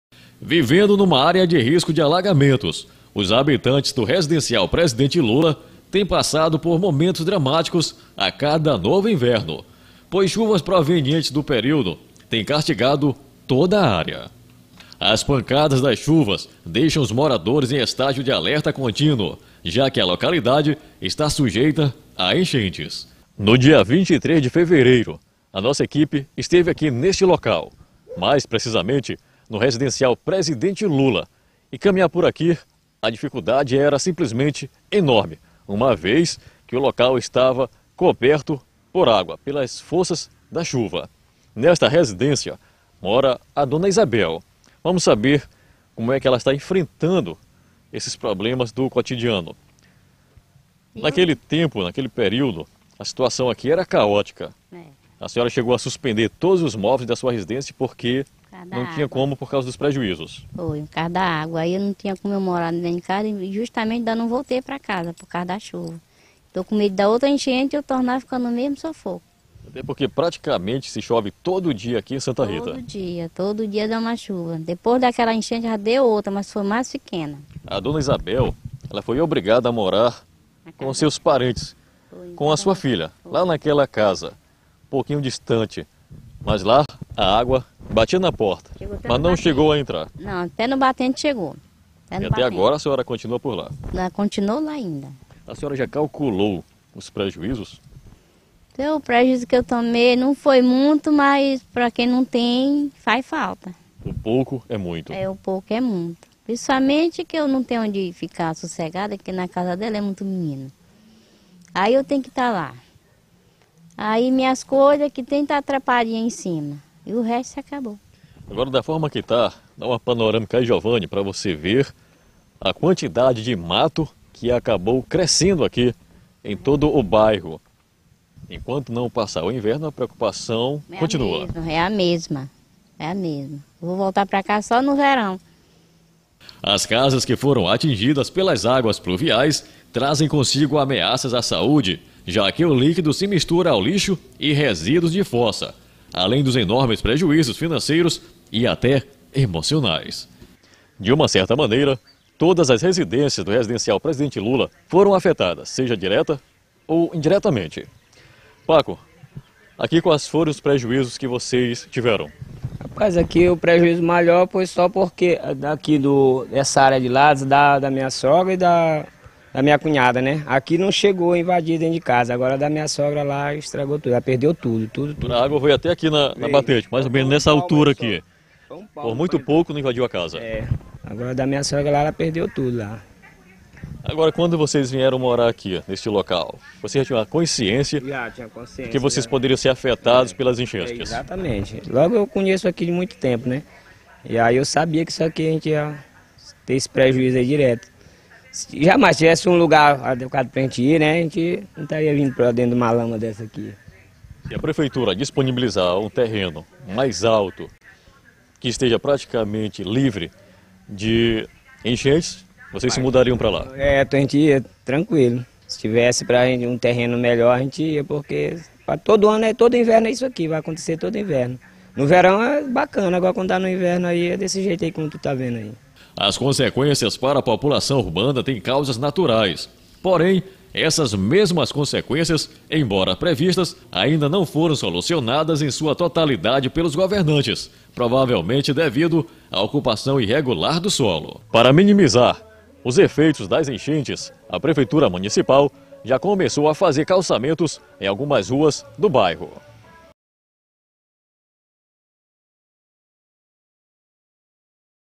RESIDENCIAL LULA EM SANTA RITA -MA
OS PROBLEMAS NO RESIDENCIAL LULA EM CAREMA CONTINUAM SEM SOLUÇÕES